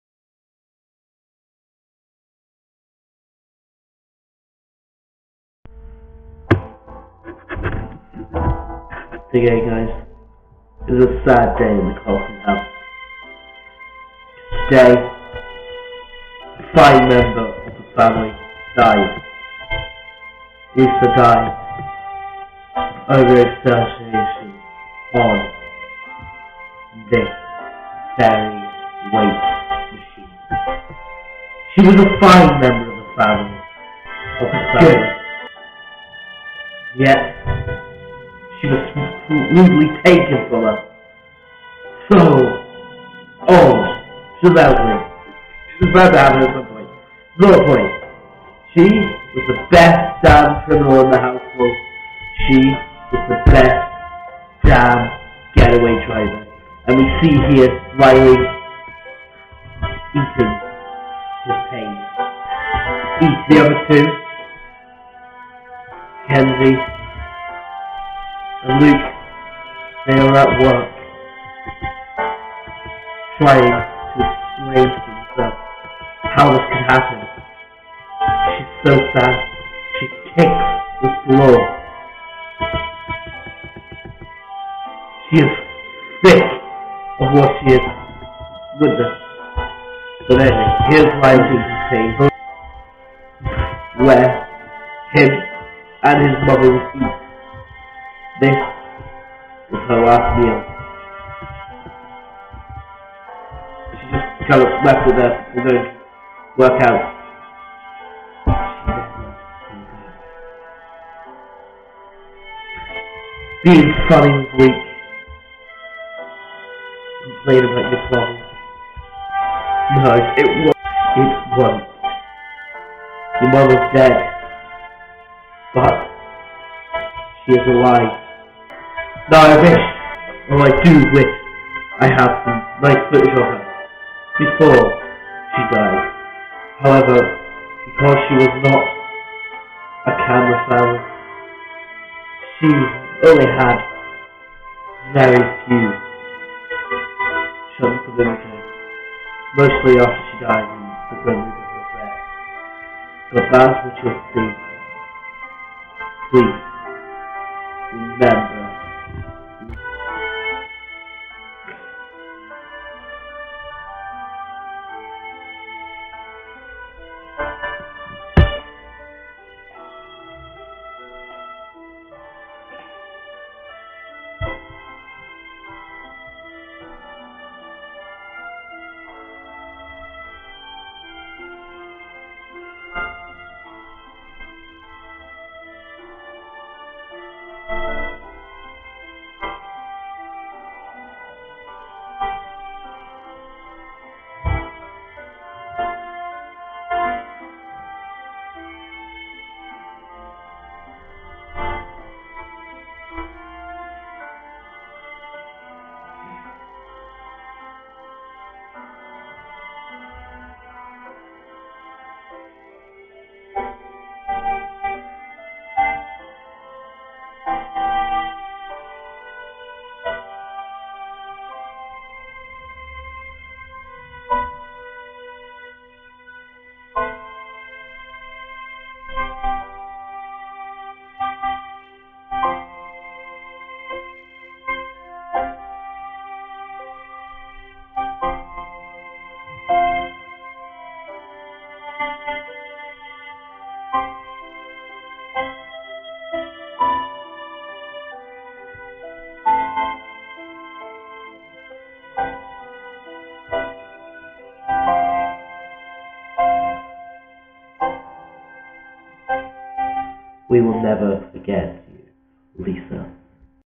The death of Lisa Cocharam
Today a member of the cocharam family died a fine woman,mother,lover and criminal. Good bye Lisa